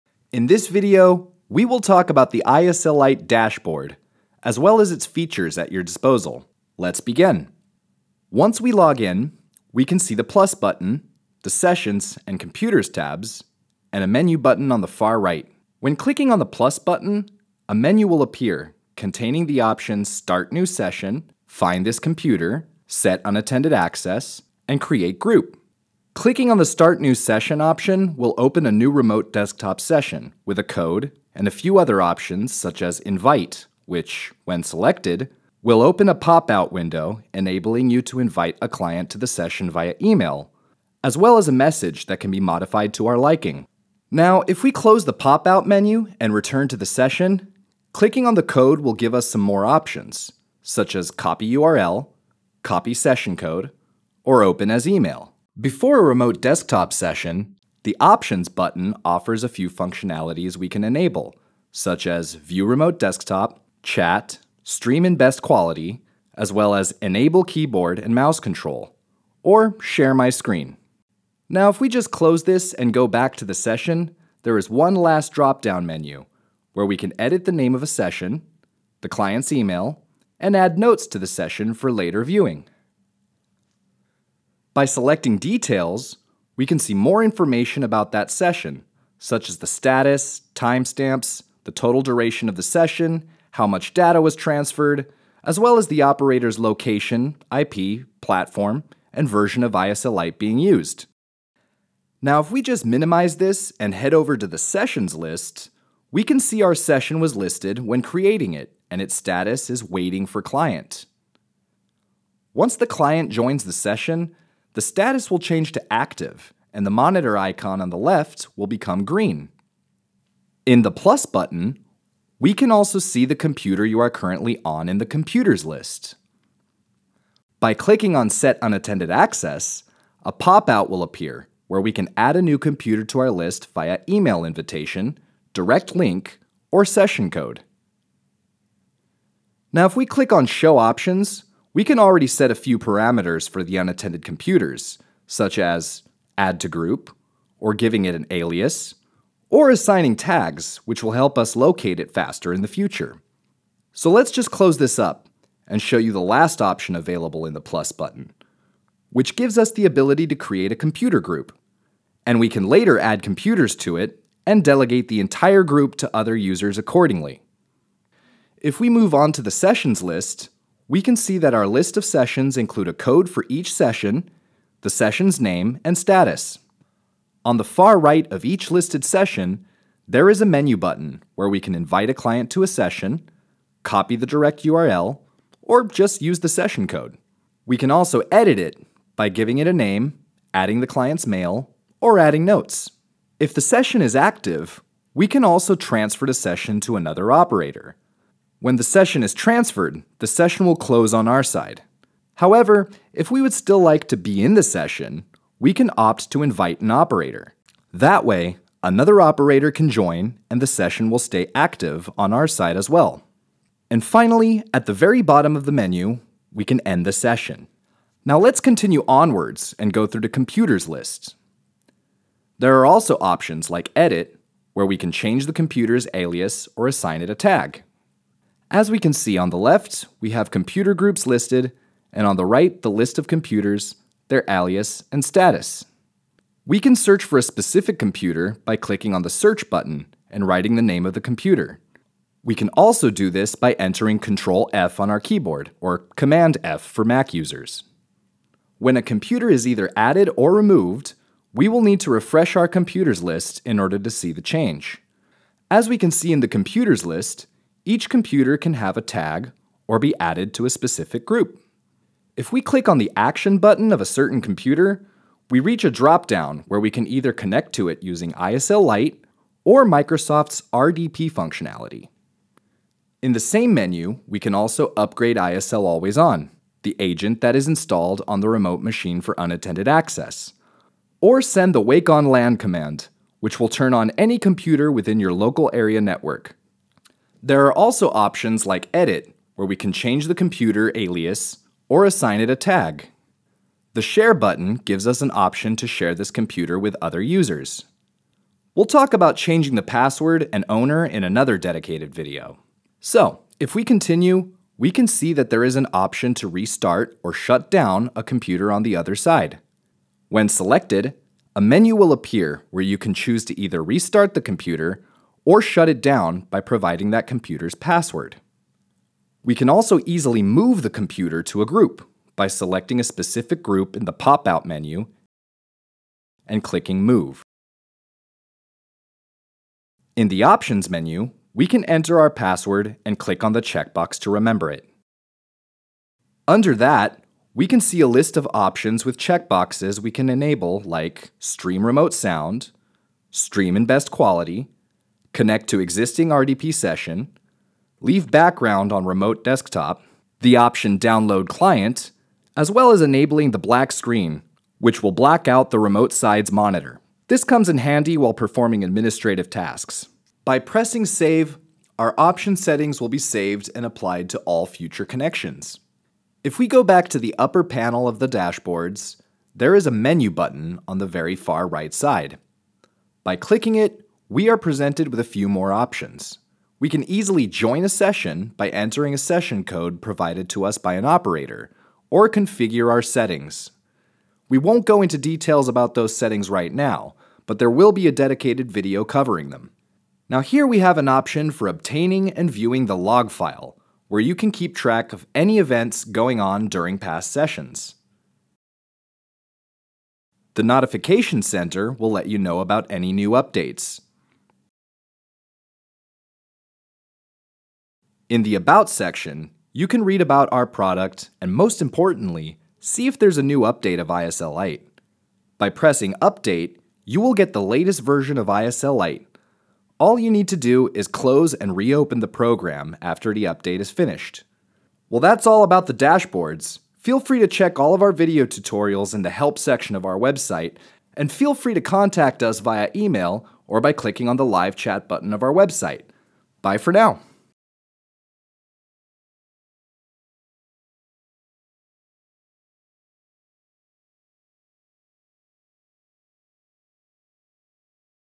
How to access the most important features | ISL Online Remote Desktop
In this video, important features of ISL Online remote support software are presented, such as:
🔸 how to start a new remote support session,
🔸 functionalities you can enable before starting the remote session,
🔸 information about the remote session,
🔸 the list of your remote computers,
🔸 setting unattended access to a remote computer,
🔸 transferring the remote support session or inviting another operator to yore remote support session,  and
🔸 updating remote support software to the newest version.

Start a free 15-day trial with all the whiteboard tools and other features included. No credit card required ➡️

00:00 Dashboards
00:25 Start New Session
01:24 Information about the session
02:07 Set Unattended Access
02:50 Sessions list
03:13 Transfer the session or Invite an Operator
03:37 Computers' list
05: 04 Restart/Shutdown
06:30 Log File
06:48 Update to the newest version